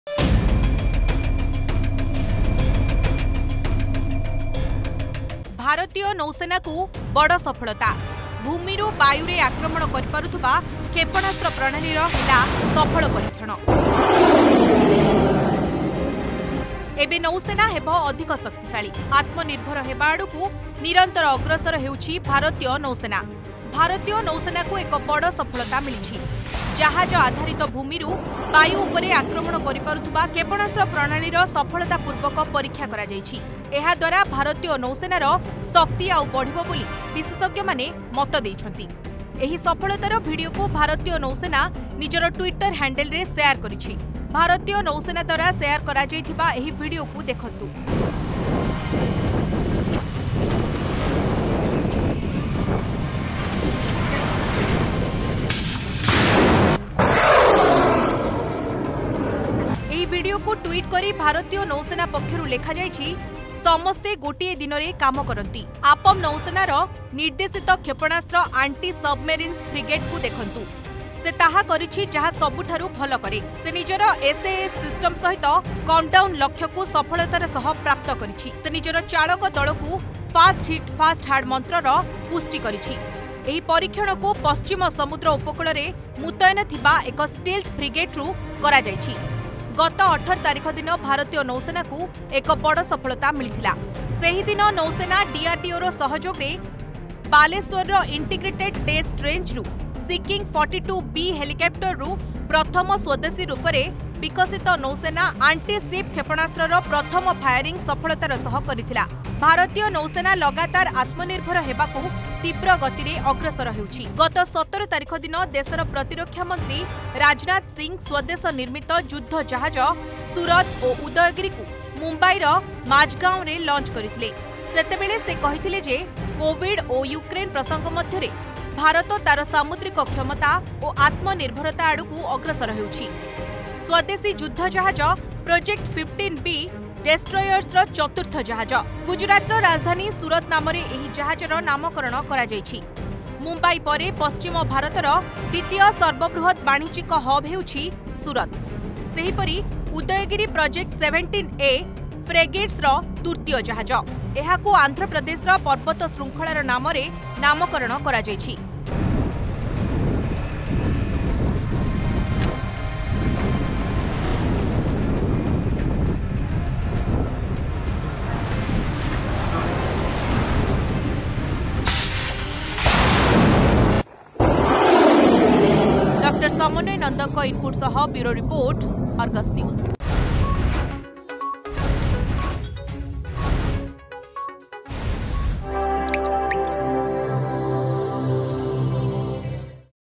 Indian Navy successfully tests surface to air missile system from warship
The Indian Navy successfully tested its ship-based surface to air missile system by taking down a low flying target.